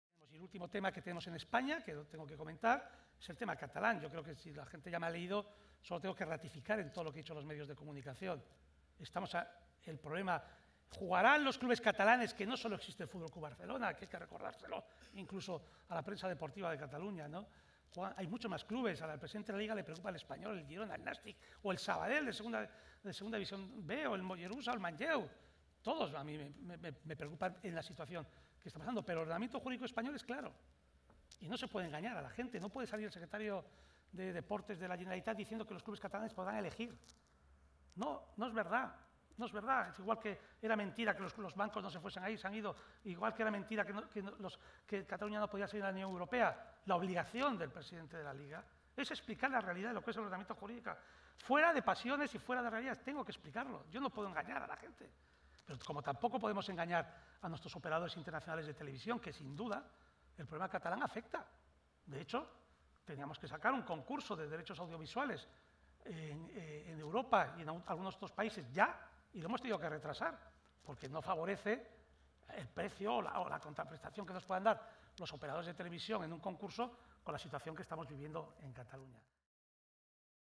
Tebas: "El problema de Cataluña puede afectar mucho a nuestra Liga" | Diario AS
El presidente de LaLiga, Javier Tebas, advirtió de los perjuicios que le está ocasionando a la Liga el conflcto de Cataluña durante su intervención en el World Football Summit: "Ratifico lo que he dicho sobre todos los clubes catalanes, no sólo el Barça. El reglamento jurídico español es claro. No se puede engañar y decir que los clubes podrán elegir dónde juegan. La obligación del presidente de LaLiga es explicar el reglamento. No puedo engañar a la gente ni a los operadores internacionales con los derechos. Hemos tenido que retrasar el concurso que solemnos hacer por estas fechas hasta saber qué pasará en España. Estamos en medio de un problema que puede afectar muchísimo al valor de nuestra competición, aunque no creo que ocurra. LaLiga perdería en torno a un 20% de ingresos si se van el Madrid o el Barcelona. Ayer leí que Roures dijo que no entendía LaLiga sin el Barça o el Madrid, y algunos no entendemos España sin Cataluña. Confiamos en que el Gobierno de solución".